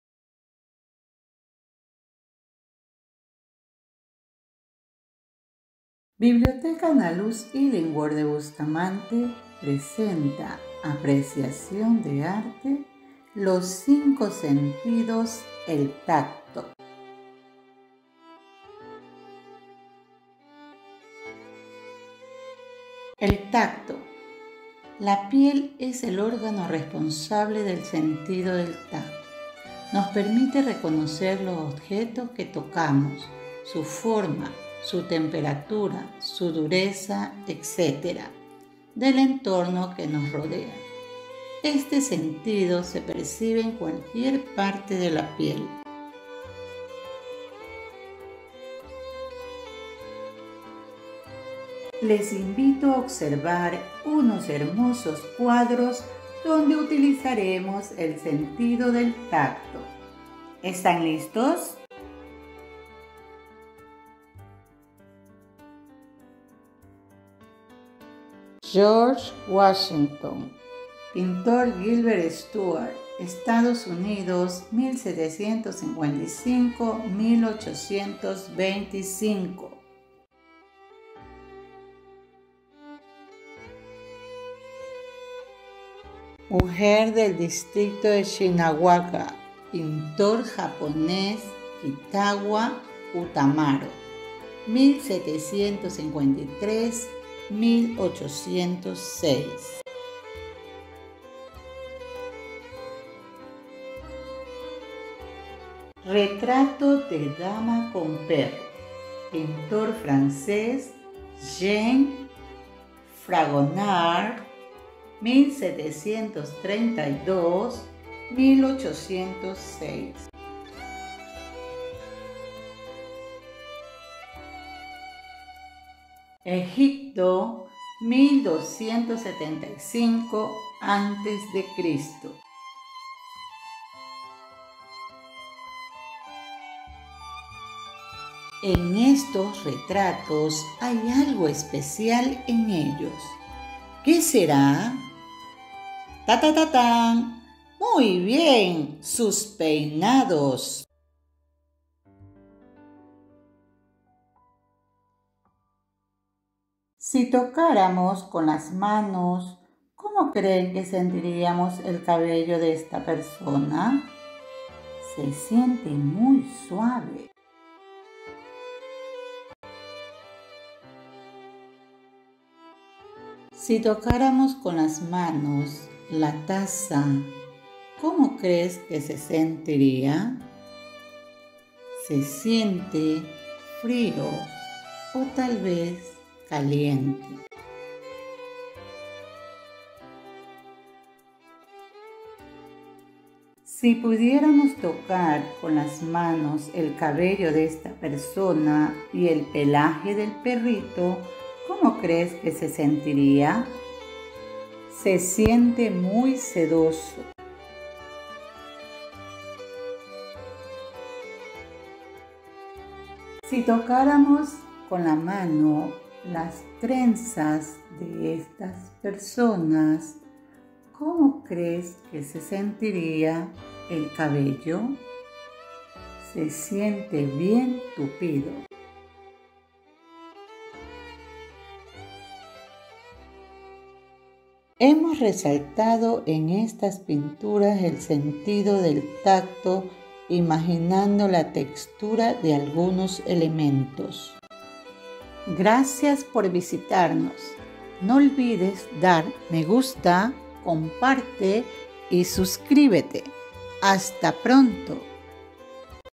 Una mirada al arte a través de los sentidos: El tacto
Una forma fácil y sencilla de imaginarnos  la textura de algunos elementos.
Para 3er y 4to año básico